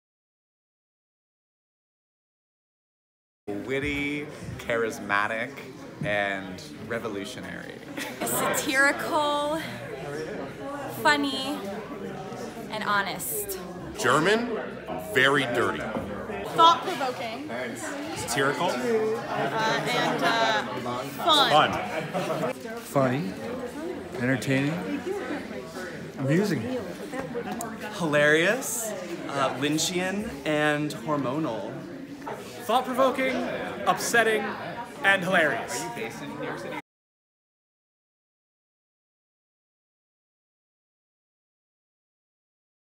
Three Words to Describe Testosterone at The Cherry
Rambo meets Pee-wee's Playhouse in an outrageous new dark comedy from Germany.

The fourth Cherry Artists' Collective production of our 2018/19 season, Testosterone by Germany’s Rebekka Kricheldorf, runs  February 21–March 3. This pitch-dark fable hilariously and unsparingly probes both toxic masculinity and the limits of liberal do-goodery in extreme times. Has seen productions in Germany, Argentina, Guatemala, Mexico, Colombia and Venezuela: the Cherry presents Testosteron's English-language première.

“Fast-paced antics and multi-layered satire will not disappoint” – nachtkritiken (German)

WITH:
Eric Brooks*
Helen Clark*
Lucy Gladstone
Karl Gregory*
Benno Ressa
AJ Sage
David Studwell*

(*member, Actors' Equity Association)

At the Cherry Artspace: 102 Cherry Street, Ithaca NY

Content Warning: Testosterone is a very dark and satirical play that contains numerous elements that may be triggering to some patrons.

Co-Producers:
Donald Spector & Stacia E. Zabusky

Media Underwriter:
Cinemapolis

Underwriter:
Ithaca Hilton Garden Inn